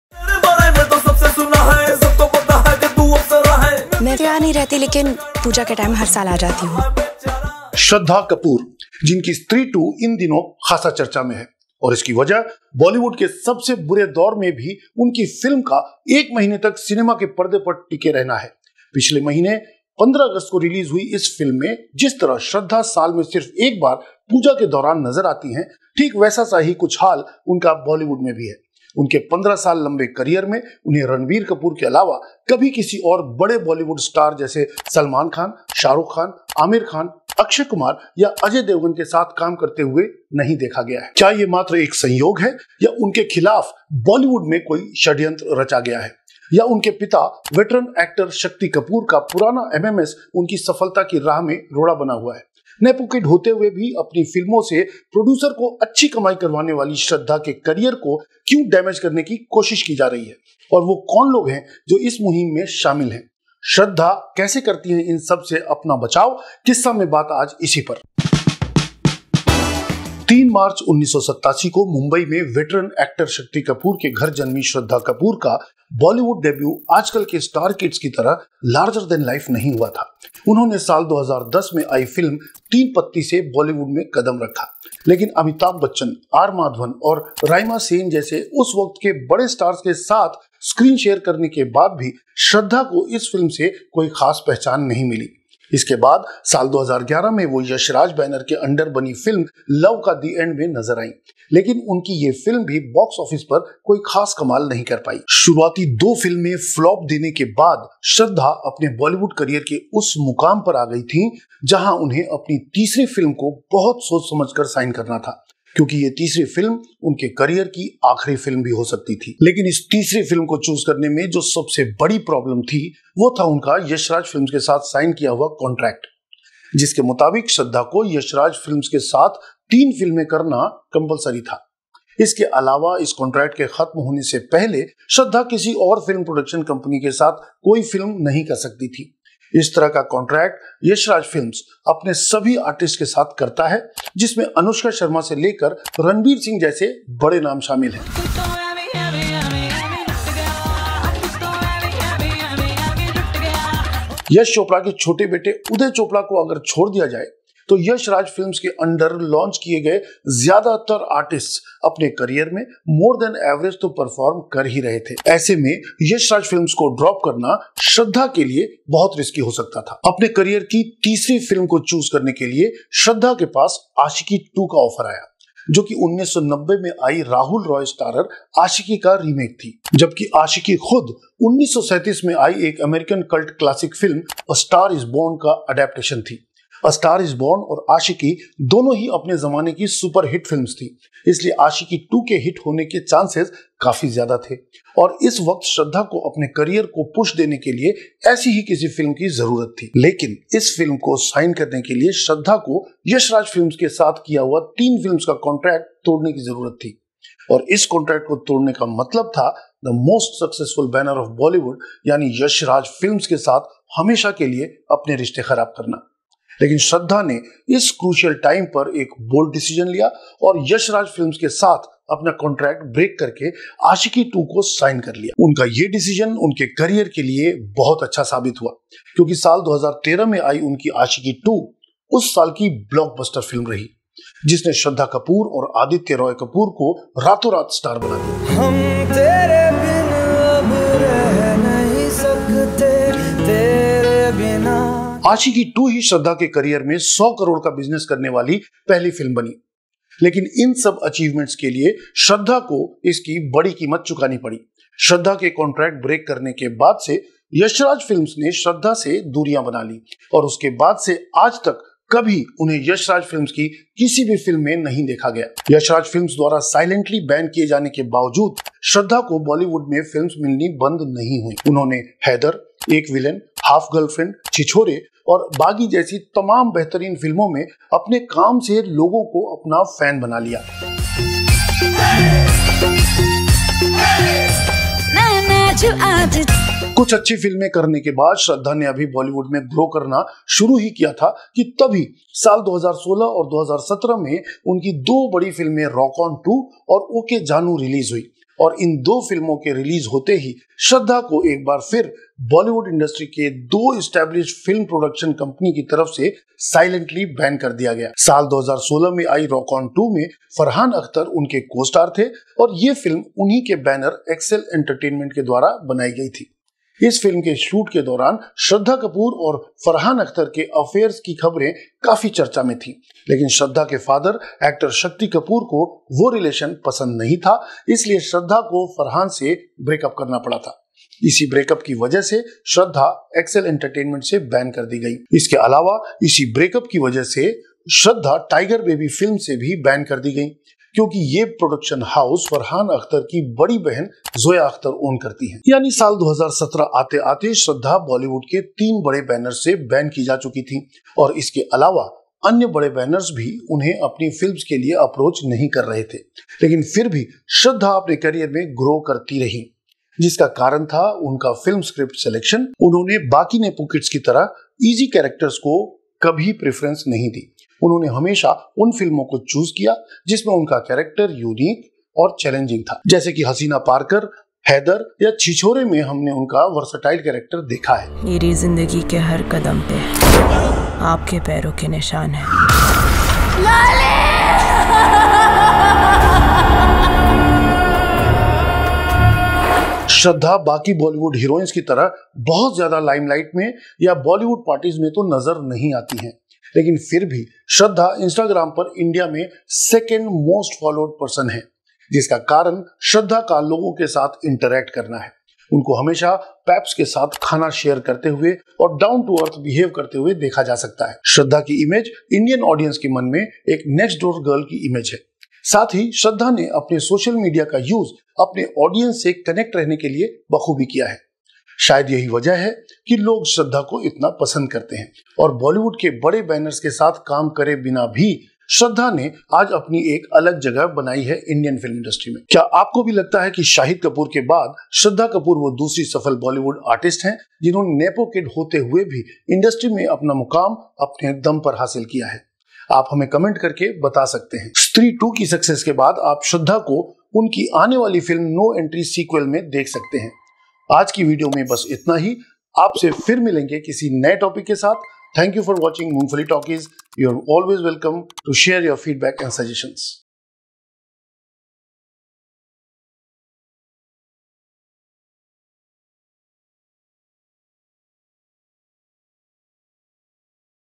Shraddha Kapoor Biography | EP- 40 | Kissa | Rahul Mody | Stree 2 | Nepo Kid
In this insightful video, we explore the remarkable journey of Shraddha Kapoor, who faced significant challenges after being banned by several prominent film production companies. Despite these setbacks, she has managed to carve out a successful career in the Indian film industry. Join us as we delve into her resilience, determination, and the strategies she employed to overcome adversity and achieve her dreams. Discover the lessons we can learn from her inspiring story.